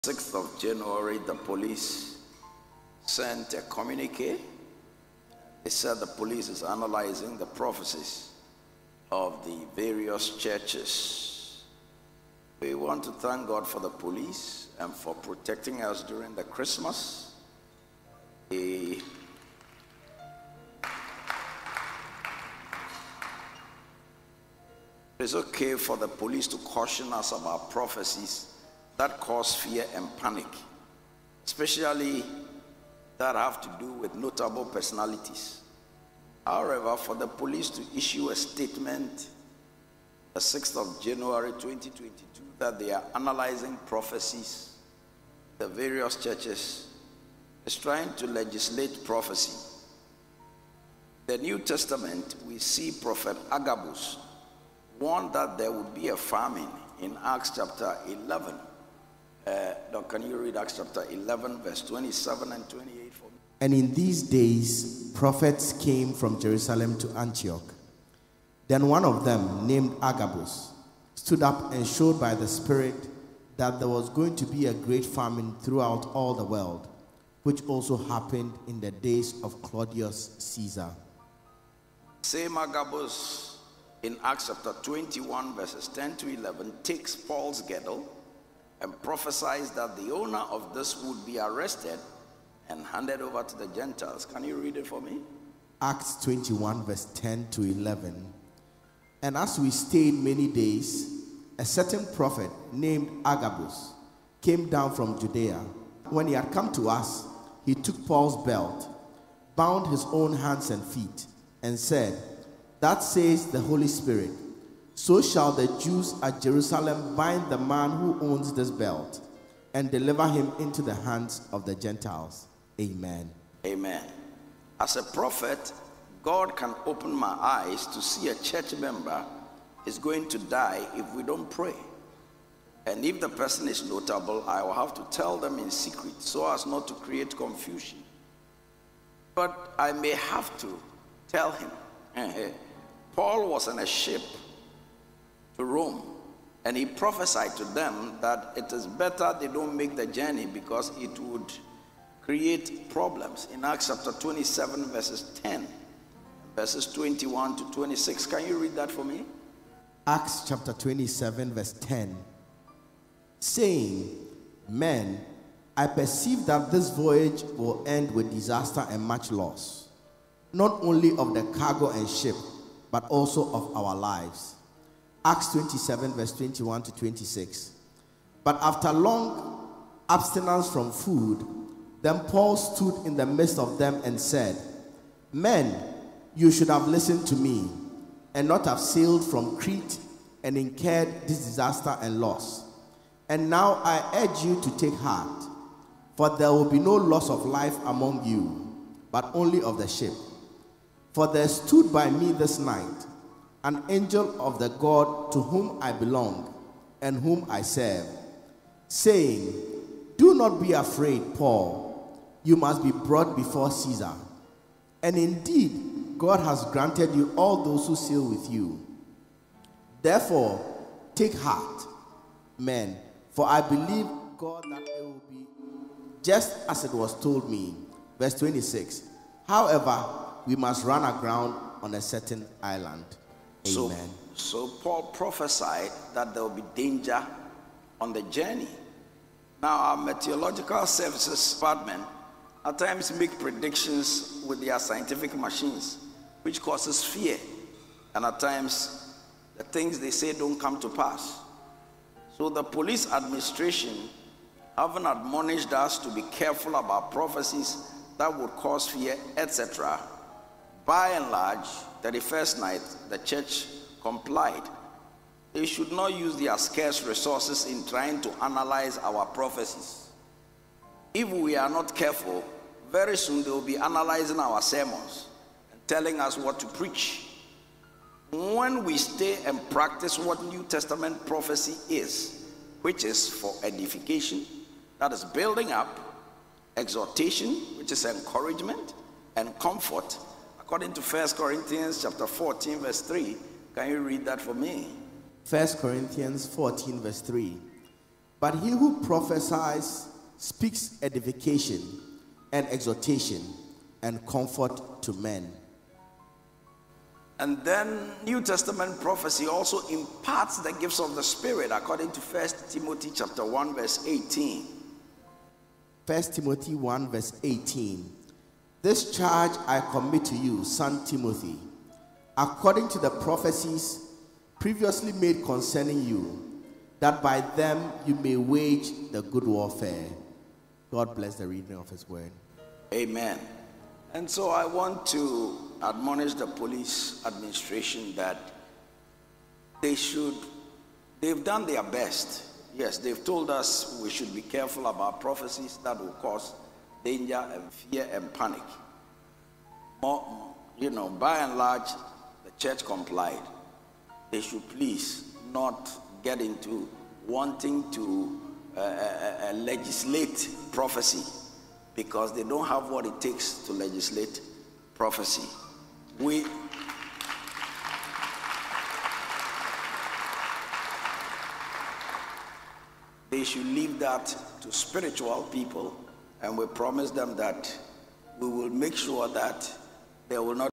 Bishop Charles Agyinasare cautioned ⚠️ Ghana Police
Ghana police should not Legislate Prophecy